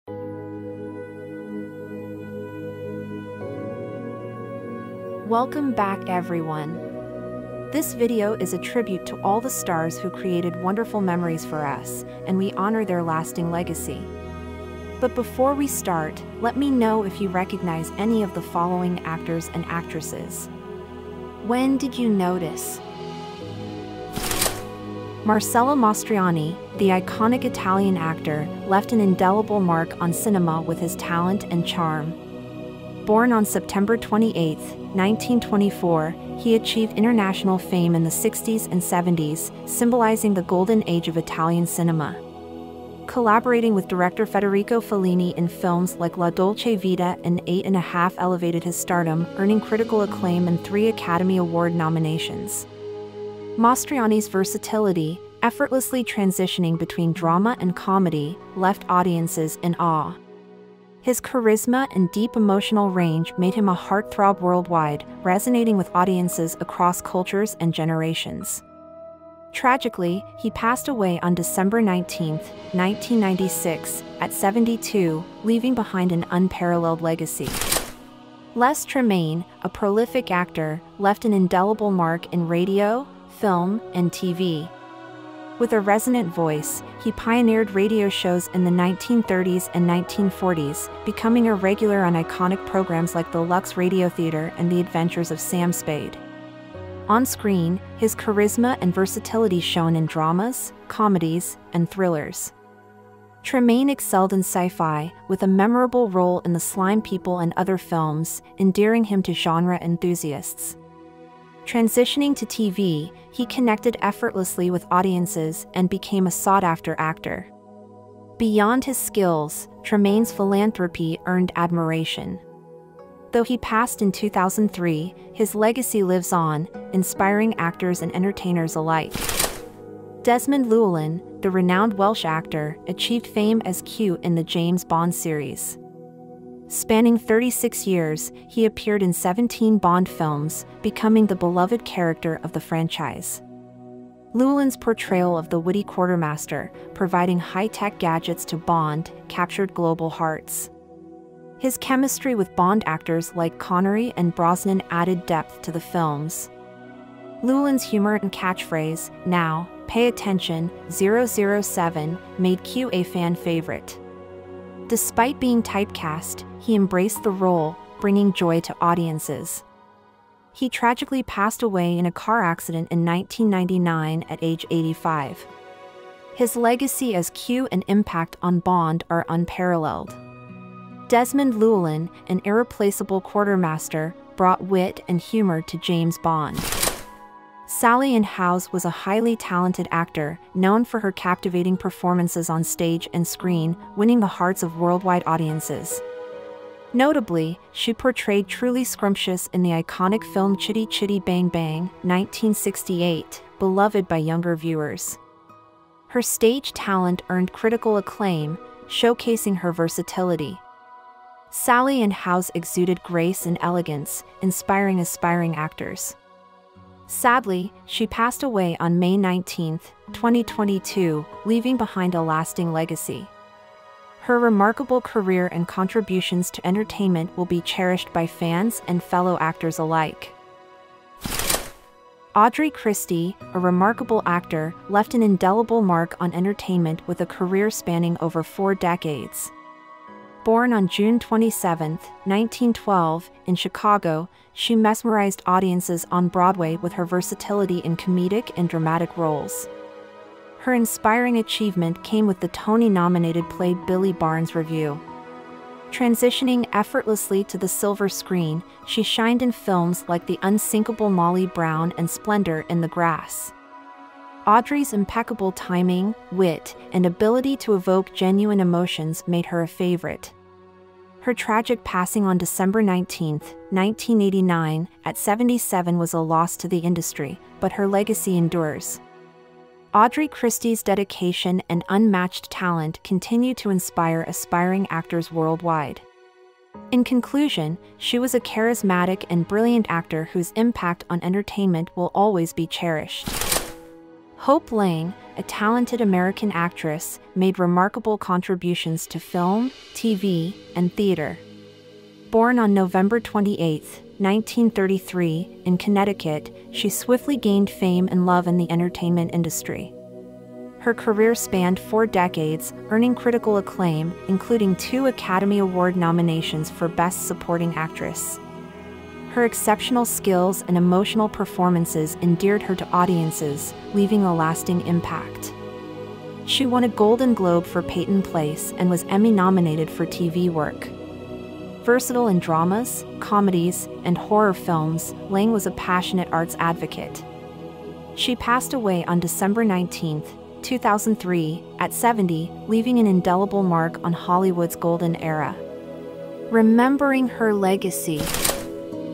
6 Famous Celebrities who died today 19th December - remembering big stars - 2023
Les Tremayne
Desmond Llewelyn
Hope Lange
Sally Ann Howes
Audrey Christie
Marcello Mastroianni